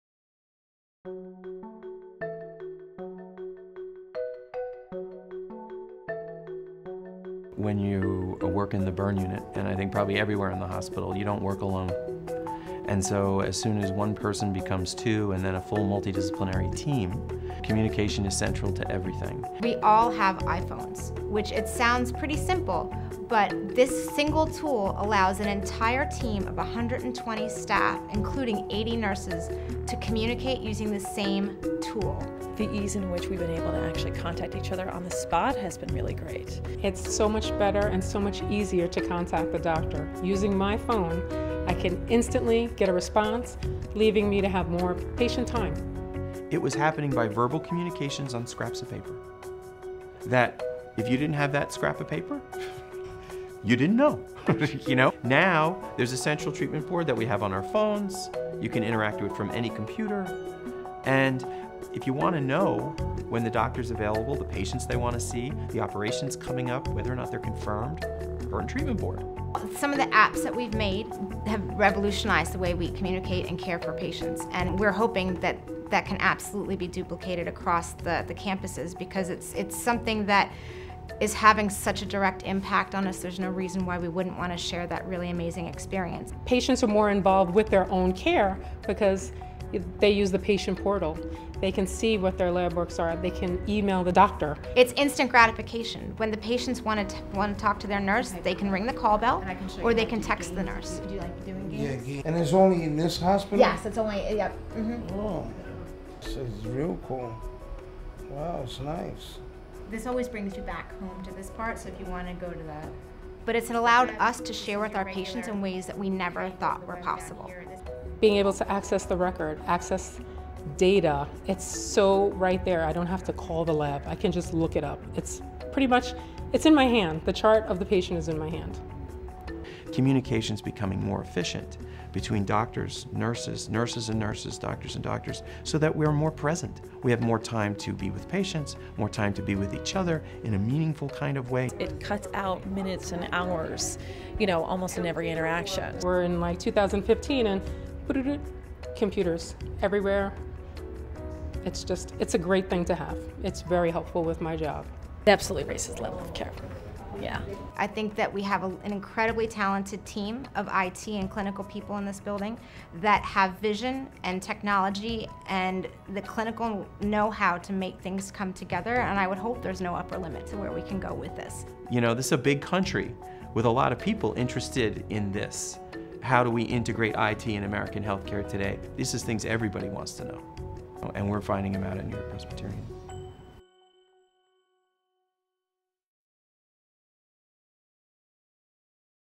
First iUnit at NewYork-Presbyterian Hospital
The iUnit Initiative, focused on creating the patient care unit of the future at NewYork-Presbyterian, launched September 2014 in the William Randolph Hearst Burn Center at NYP/Weill Cornell Medical Center.

The Hearst Burn Center works directly with NYP's Innovation Center and IT to develop and test technologies focused on mobility, patient engagement, collaboration, and quality., with the hopes of scalability throughout the entire hospital.